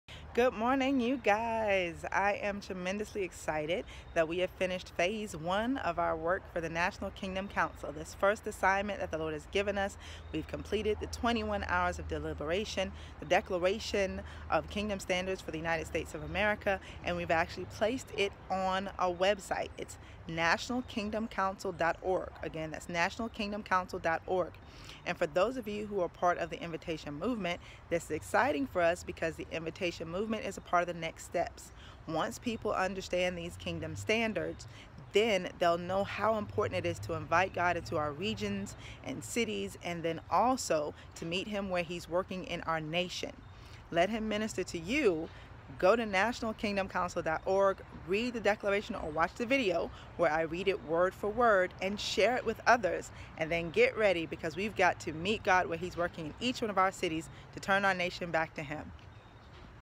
Now our work begins!!!
The Declaration of Kingdom Standards for the United States of America has been published! To hear the entire Declaration read aloud or download the PDF, During the 10 Days of Prayer, the Lord told me to convene a National Kingdom Council, which is a gathering of believers to discern the will of God for our nation. This was essential, because the day after the Return, God said to me, “My people don’t know how to return to Me.” He made it very clear that the cause of this is failure of the Body of Messiah to unite and declare His standards to the world. In the National Kingdom Council, we came together from both genders, various ages, ethnicities and denominations. We searched the Scriptures and stood in His divine council individually. However, for 7 nights, we convened for 3 hours each night to discern the fullness of God’s will on 7 important issues, as we stood in His divine council together. The Declaration includes all of the revelation from 21 hours in the Presence of God, seeking Him on the following 7 issues
The Bill of Rights
Women's Rights & Abortion
Family Values & Gay Rights
Race & Social Equality
Foreign Policy & Immigration
Criminal Justice & Law Enforcement
Economics & Poverty
The Invitation Movement will be an important part of the next steps for the Standard Bearers who agree with the Declaration, because people who understand God’s standards for our nation will be drawn to invite Him into their cities and communities. Be very prayerful in this season, as we come together to accept God’s invitation to meet Him where He’s working in our nation and lead other Americans in this move of God, one city at a time. For more information, to download the PDF, to sign our petition to share this declaration with our national leaders, for additional resources or to join us in taking action next steps, Let’s turn our nation back to God!